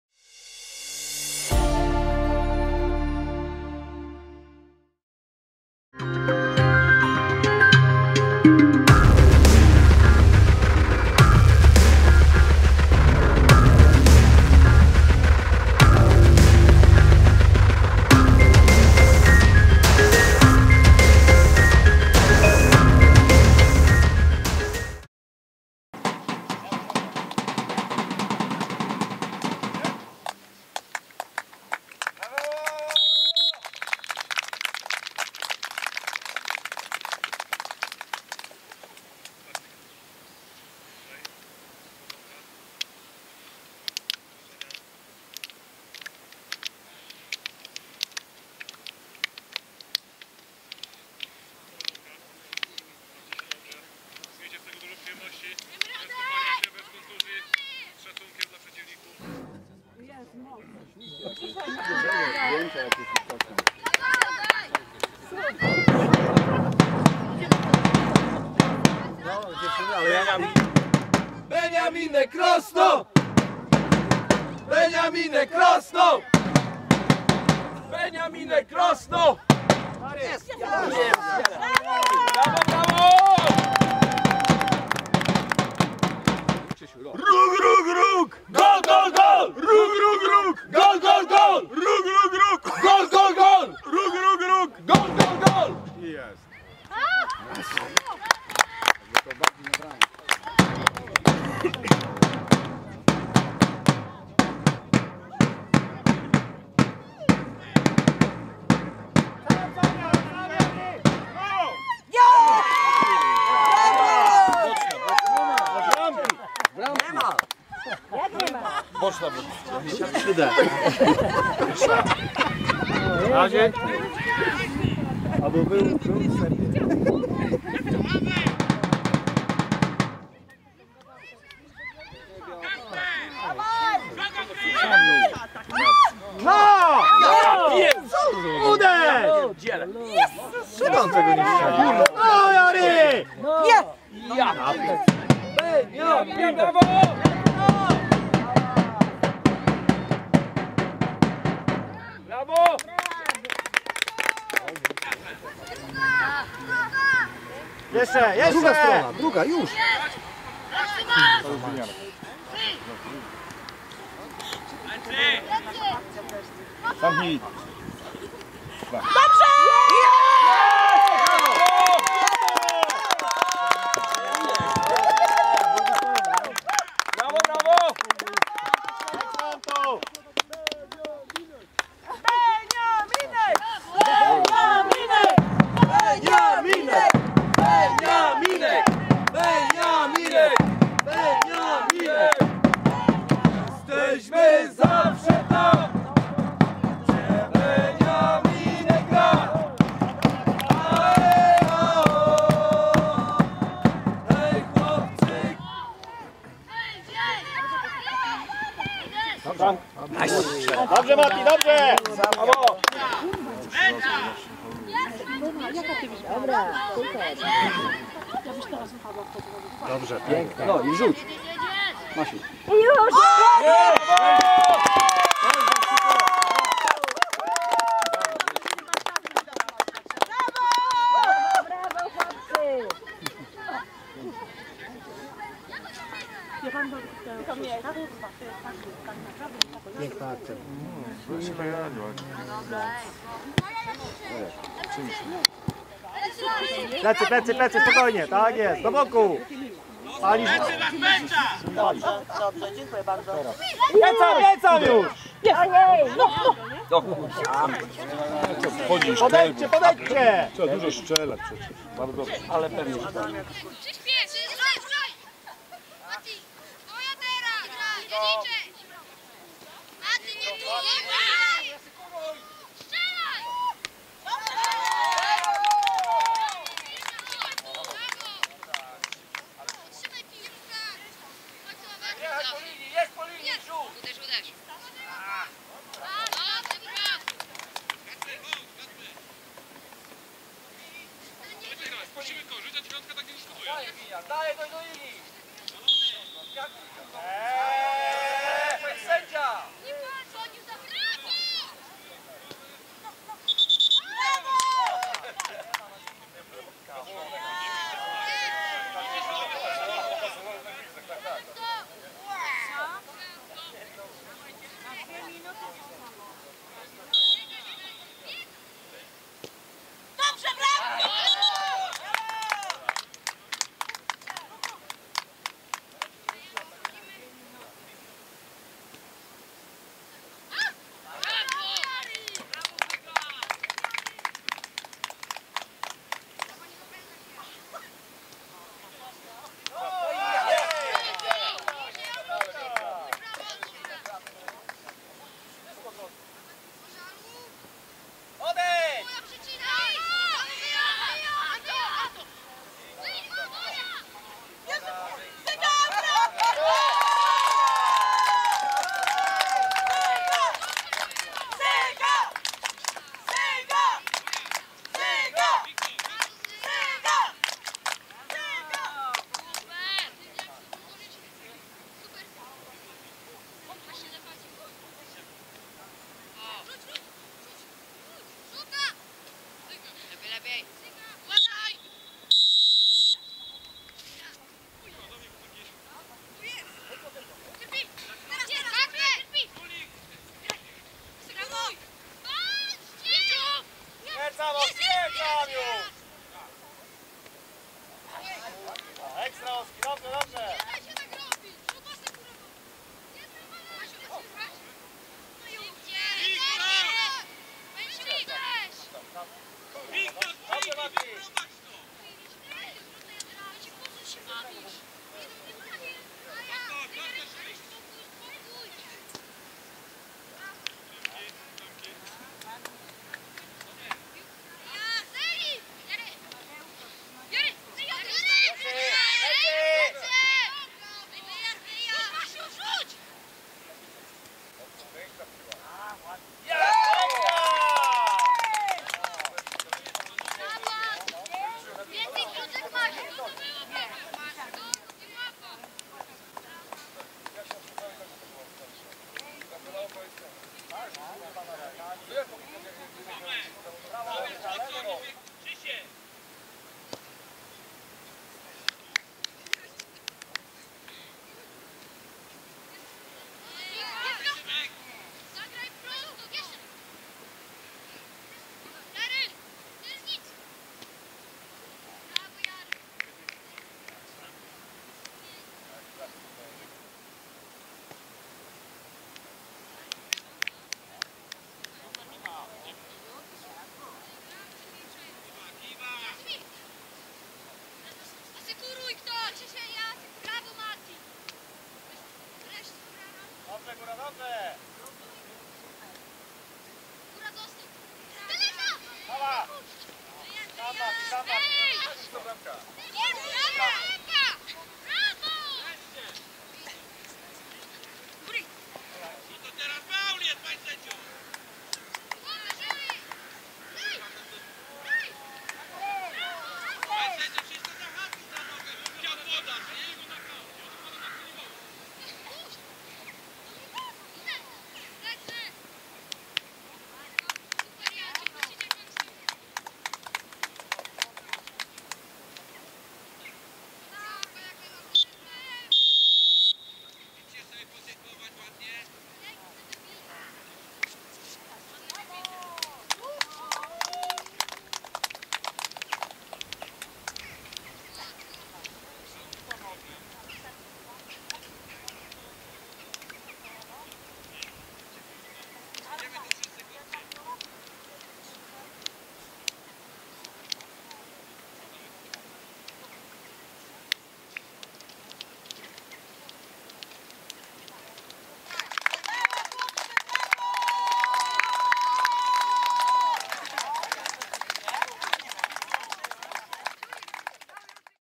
Młodzik Starszy: Beniaminek Krosno - Karpaty MOSiR Krosno
W Podkarpackiej Lidze Młodzika Starszego doszło do derbów Krosna. Beniaminek Krosno U-13 pod wodzą Karola Dobrzańskiego pewnie pokonał zespół Karpat, a dwa gole zdobył Antoni Kozubal. Warto dodać, że bramkostrzelny pomocnik gospodarzy i pięciu jego kolegów zostało powołanych do Kadry Podkarpacia w roczniku 2004. Wynik spotkania mógł być wyższy, ale kilkukrotnie bramkarz Karpat Tomasz Łapka świetnie bronił strzały przeciwnika.